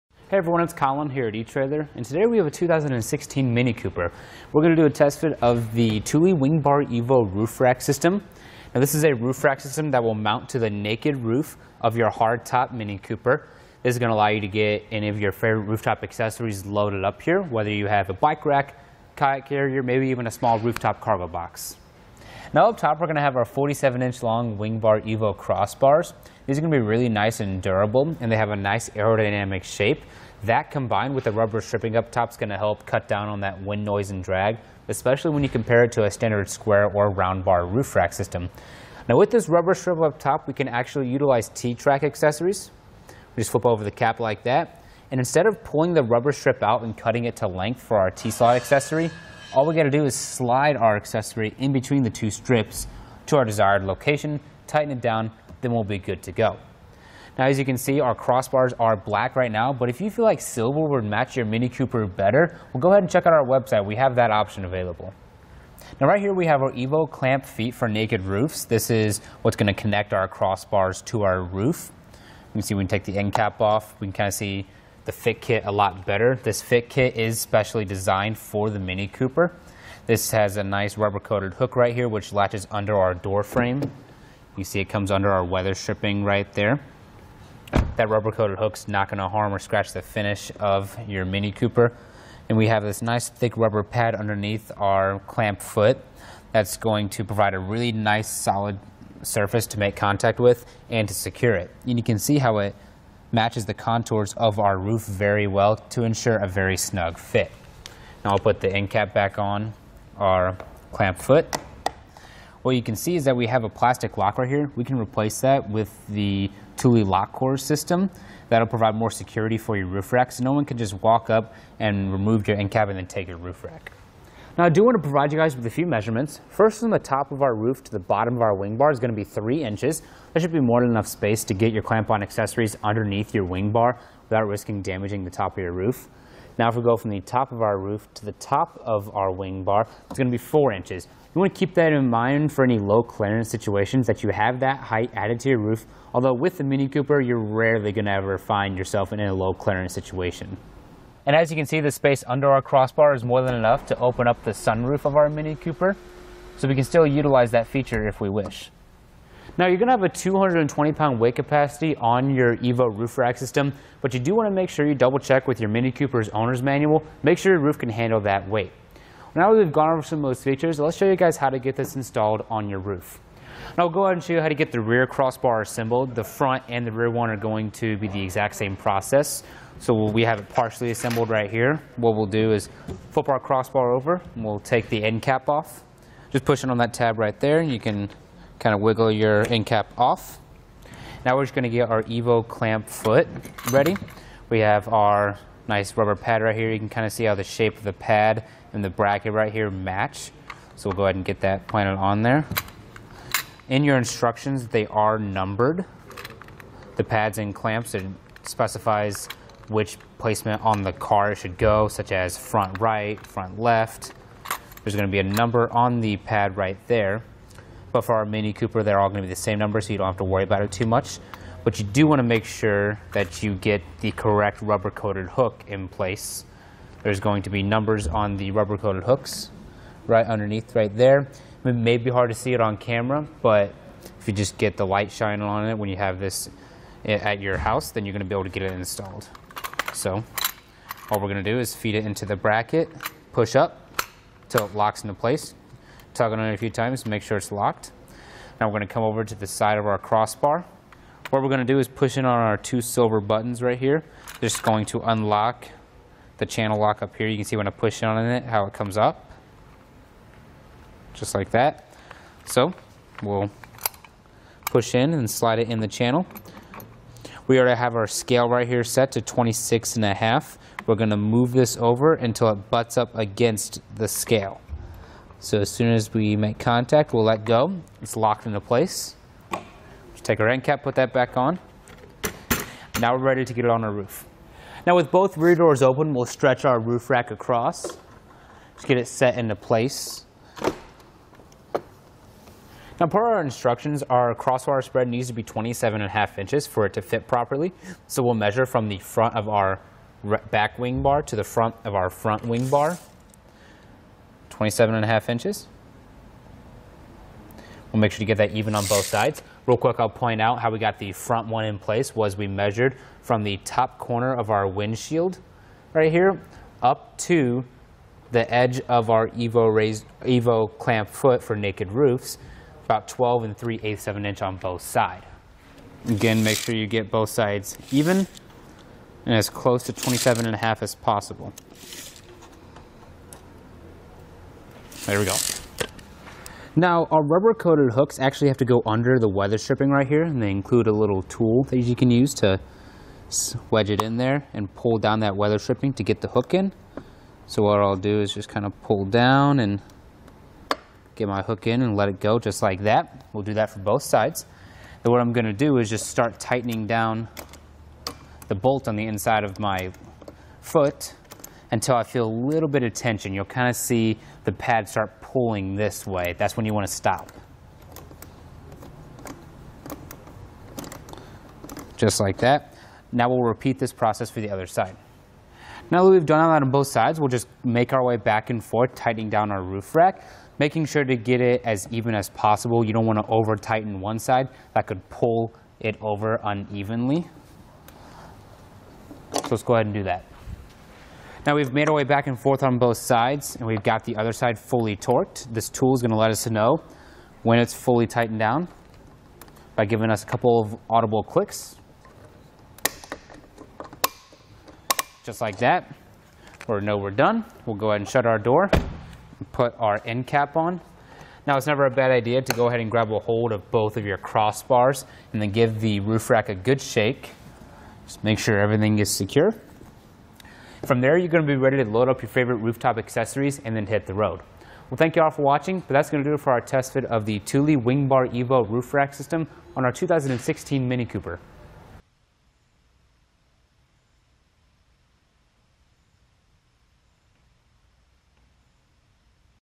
2016 Mini Cooper Fit Kit for Thule Evo Clamp and Edge Clamp Roof Rack Feet: Complete Installation
Colin:                                                 Hey everyone. It's Colin here at Etrailer, and today we have a 2016 Mini Cooper. We're going to do a test fit of the Thule Wingbar Evo Roof Rack System. Now, this is a roof rack system that will mount to the naked roof of your Hardtop Mini Cooper is going to allow you to get any of your favorite rooftop accessories loaded up here, whether you have a bike rack, kayak carrier, maybe even a small rooftop cargo box.Now up top, we're going to have our 47-inch Long Wing bar, Evo crossbars. These are going to be really nice and durable, and they have a nice aerodynamic shape. That combined with a rubber stripping up top is going to help cut down on that wind noise and drag, especially when you compare it to a standard square or round bar roof rack system.

Now, with this rubber shrivel up top, we can actually utilize T-track accessories.We just flip over the cap like that, and instead of pulling the rubber strip out and cutting it to length for our Tesi accessory, all we're going to do is slide out accessory in between the two strips to our desired location, tighten it down, then we'll be good to go.Now, as you can see, our crossbars are black right now, but if you feel like silver will match your Mini-Cooper better, well, go ahead and check out our website, we have that option available.Now, right here, we have our Evo Clamp feet for naked roofs; his is what's going to connect our crossbars to our roof. You see when you take the End Cap off, we can kind of see the Fit Kit a lot better. This Fit Kit is specially designed for the Mini Cooper. This has a nice rubber coated hook right here, which latches under our door frame.You see it comes under our weather stripping right there That rubber-coated hook's not going to harm or scratch the finish of your Mini Cooper; and we have this nice thick, rubber pad underneath our clamp foot that's going to provide a really nice, solid surface to make contact with and to secure it; and you can see how it matches the contours of our roof very well to ensure a very snug fit.Now I'll put the End Cap back on our clamp foot. Well, you can see is that we have a plastic lock right here.

We can replace that with the Thule Lock Core System, that'll provide more security for your roof racks.No one can just walk up and remove your End Cap and then take your roof rack. Now, I do want to provide you guys with a few measurements. First from the top of our roof to the bottom of our Wingbar is going to be three inches. That should be more than enough space to get your Clamp-on accessories underneath your Wingbar, without risking damaging the top of your roof. Now, if we go from the top of our roof to the top of our Wingbar, it's going to be four inches.

You want to keep that in mind for any low-clearance situations that you have that height added to your roof. Although with the Mini Cooper, you're rarely going to ever find yourself in any low-clearance situation.And as you can see the space under our crossbar is more than enough to open up the sunroof of our Mini Cooper. So we can still utilize that feature if we wish. Now you're going to have a 220 pound weight capacity on your Evo roof rack system, but you do want to make sure you double check with your Mini Coopers Owner's Manual. Make sure your roof can handle that weight.Without even going over most of those features, let's show you guys how to get this installed on your roof.

Now I'll go ahead and show you how to get the rear crossbar assembled to the front and the rear one are going to be the exact same process. So what we have partially assembled right here. What we'll do is flip our crossbar over and we'll take the End Cap off, just pushing on that tab right there, you can kind of wiggle your End Cap off.Now we're just going to get our Evo Clamp Foot ready. We have our nice rubber pad right here. You can kind of see how the shape of the pad and the bracket right here match. So go ahead and get that planted on there. In your instructions, they are numbered; the pads and clamps and specifies which placement on the car should go such as front-right, front-left.There's going to be a number on the pad right there, but for our Mini Cooper, they're all going to be the same number, so you don't have to worry about it too much; but you do want to make sure that you get the correct rubber coated hook in place. There's going to be numbers on the rubber coated hooks right underneath there. It may be hard to see it on camera but, if you just get the light shine on it, when you have this at your house, then you're going to be able to get it installe